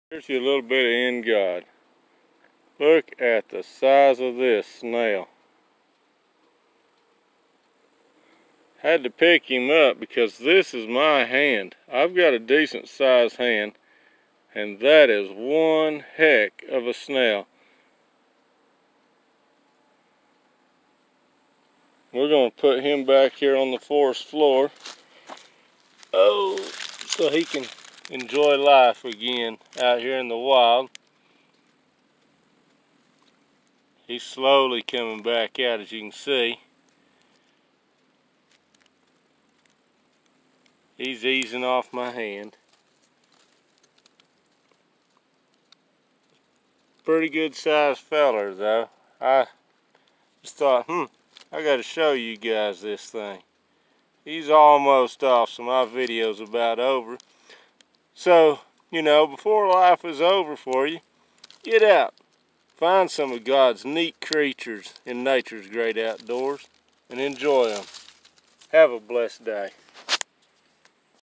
IN GOD with a BIG Snail on the move at Spring Creek “In Natures Great Out Doors"
Get out and enjoy all the animals and what God has done In Natures Great Out Doors and see the blessings you can find today. I Pray May God Bless You